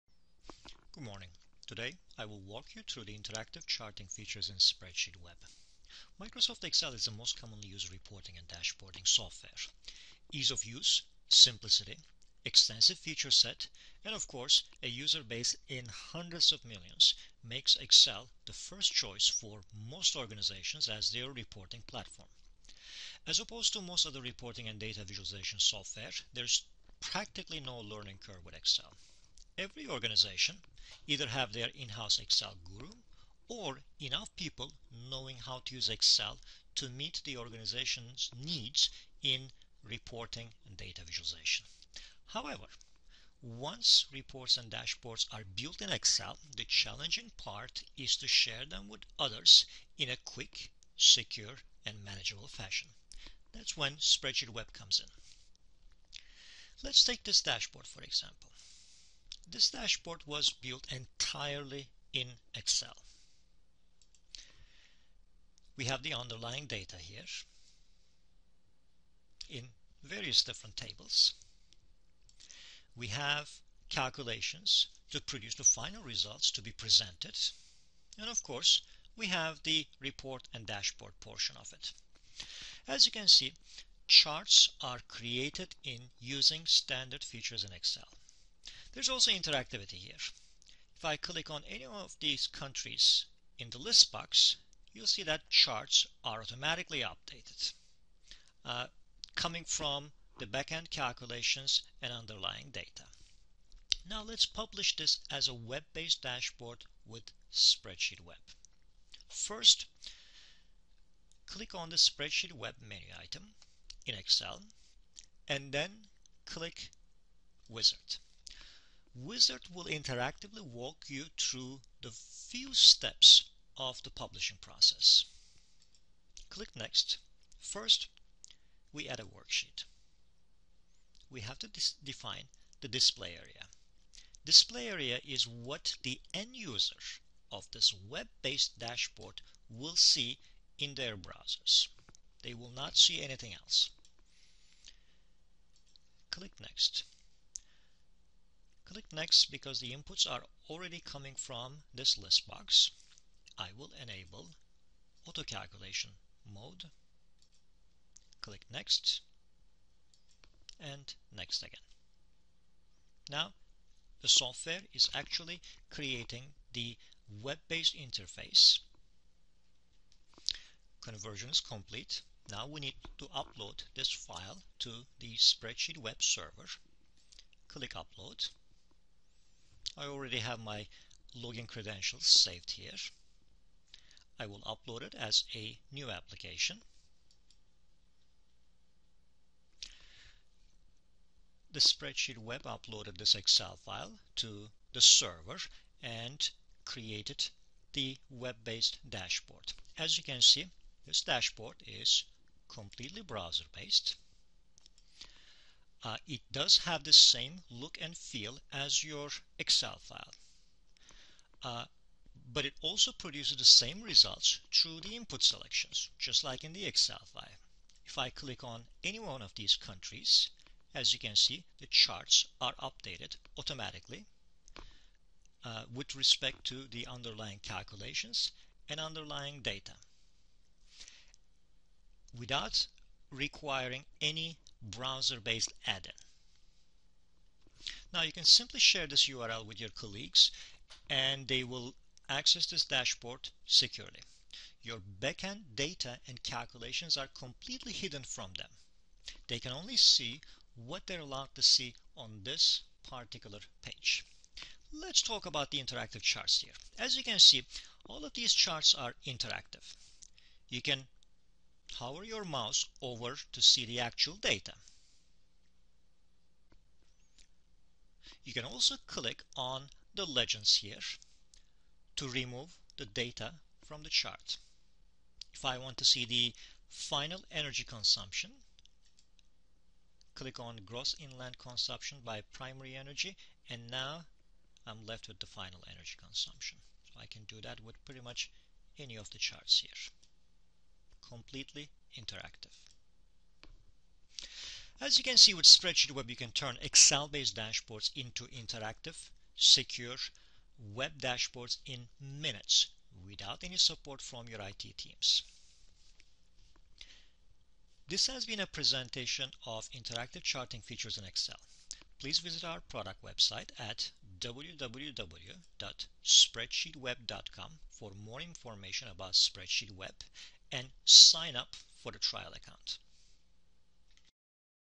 Interactive Charts with SpreadsheetWEB
This video demostrates the interactive charting features in SpreadsheetWEB which allows turning excel based reports and data visualizations into interactive web based dashboards without any programming